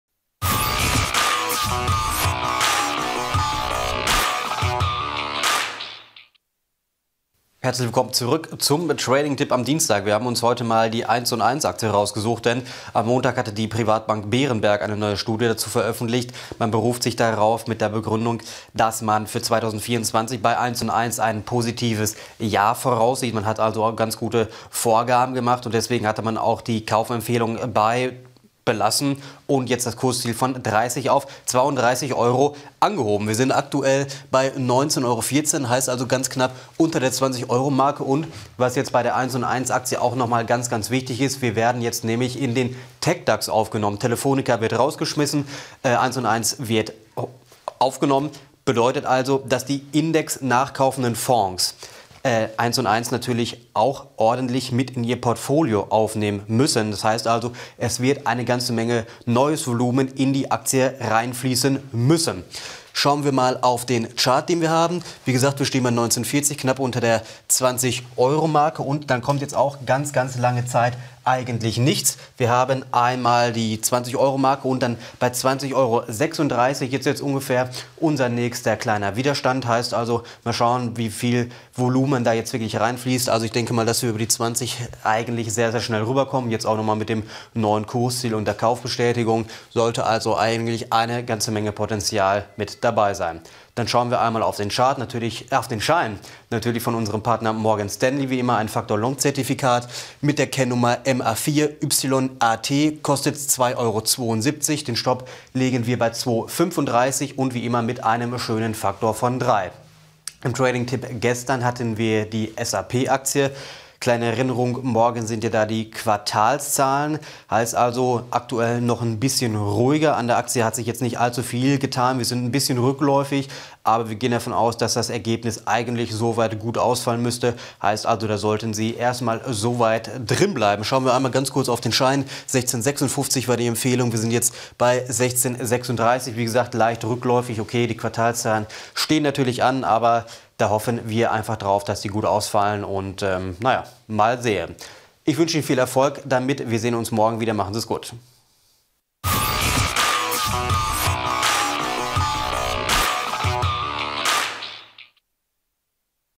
1&1 auf Kurs – Aufstieg in den TecDAX! Trading-Tipp
Gute Neuigkeiten für 1&1. Die Aktie wird durch das ausscheiden von Telefonica nun in den TecDAX aufsteigen, außerdem verleiht die Privatbank Berenberg dem Unternehmen ein neues, starkes Kursziel.

Erik Kämper mit allen Einzelheiten zum Trading-Tipp des Tages.

 / deraktionaer

Zufrieden? Bewerten Sie uns auf Google!